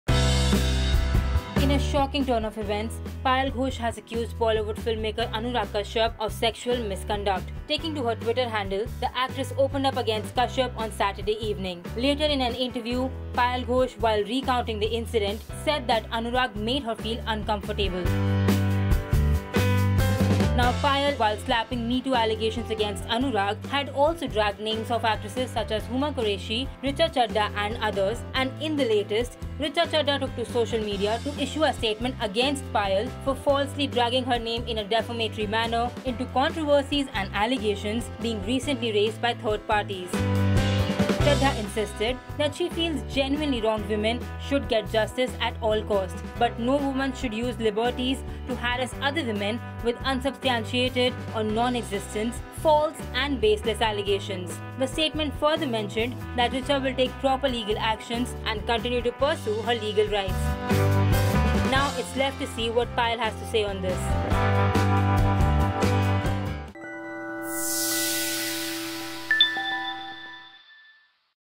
Richa Chadha to take LEGAL action against Payal Ghosh for dragging her name in MeToo controversy
Payal Ghosh who starred in the film 'Patel Ki Punjabi Shaadi' has accused Bollywood filmmaker Anurag Kashyap of sexual misconduct. Taking to her Twitter handle, the actress opened up against Kashyap on Saturday evening. Ghosh reportedly named celebrities like Huma Qureshi and Richa Chadha, others. She said Anurag told her that it was okay and all the actresses who worked with him. Now, Richa issued a statement after her name was mentioned in the allegations.